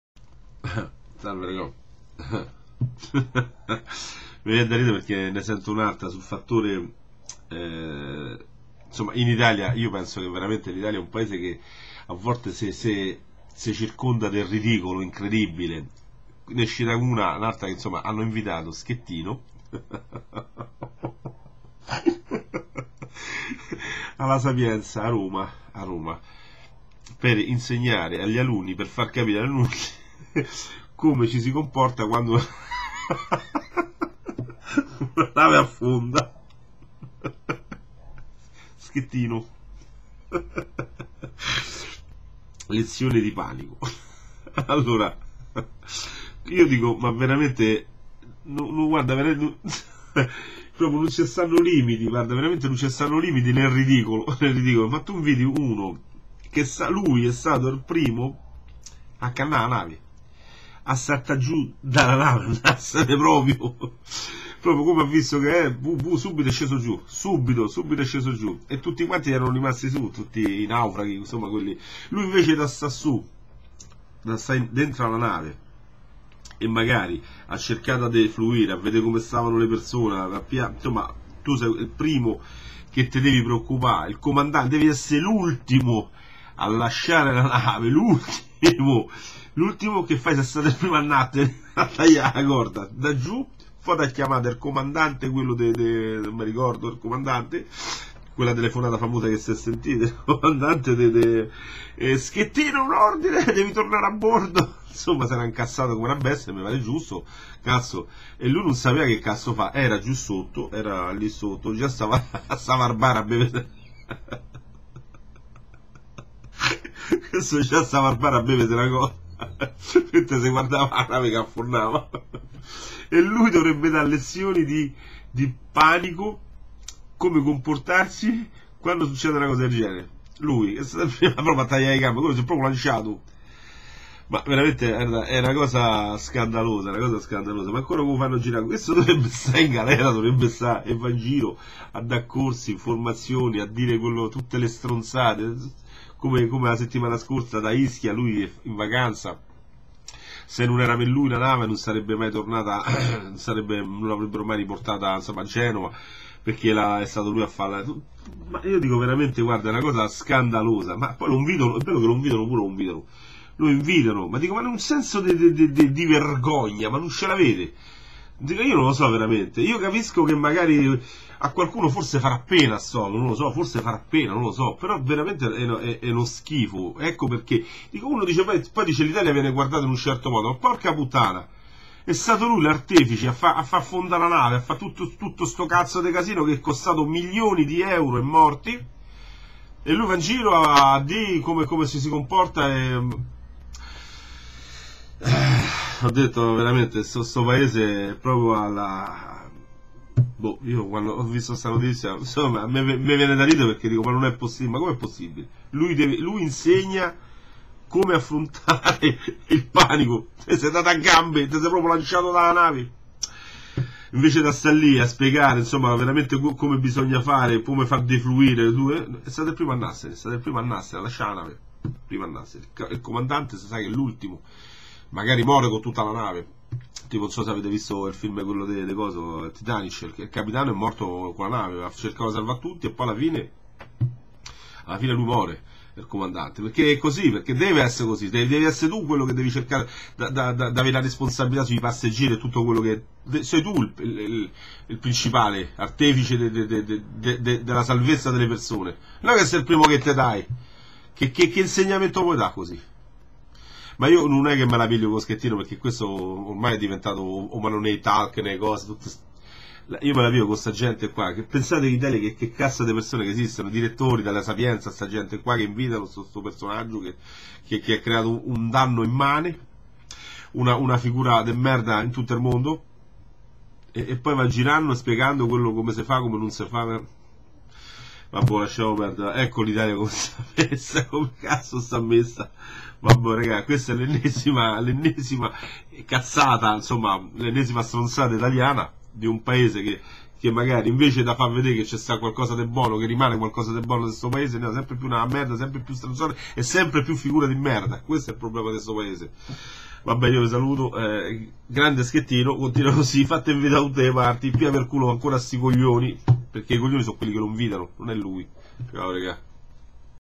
SCHETTINO OSPITE ALLA SAPIENZA DI ROMA DA LEZIONI DI PANICO
NON CI POSSO CREDERE HAHAHAHAHAHA MA E UNA BARZELLETTA ??' HAHAHAHAHA